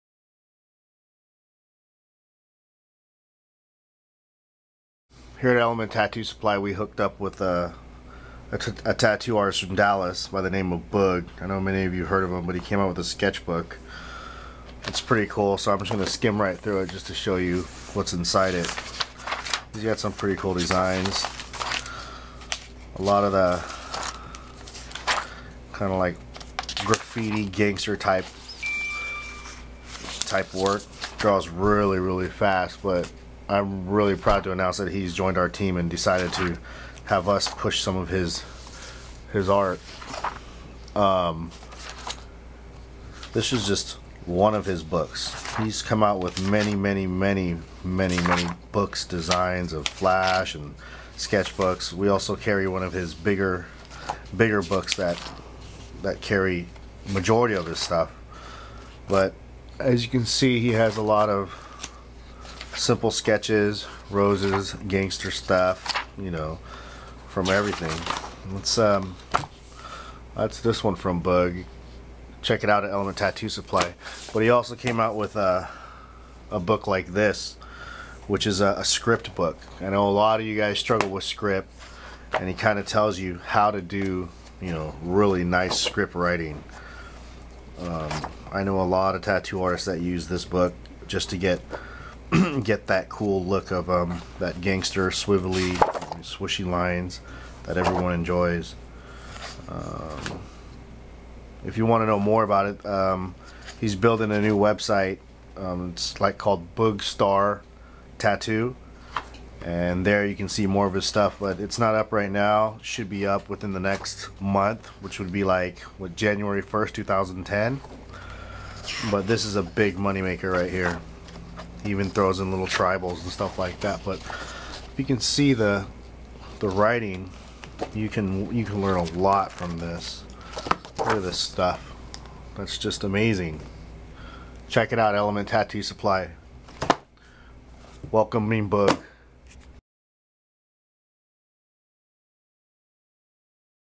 Boog Star Hooks up with Element Tattoo Supply for 2010
Element Tattoo Supply is proud to announce a new member to the family of tattoo artist;s. Boog many may have heard of mister BOOG and many of you have seen his awsome art work in tattoo shops around the world and now for 2010 Boog comes to sell his art. This is a quick Clip of one of his Sketchbooks.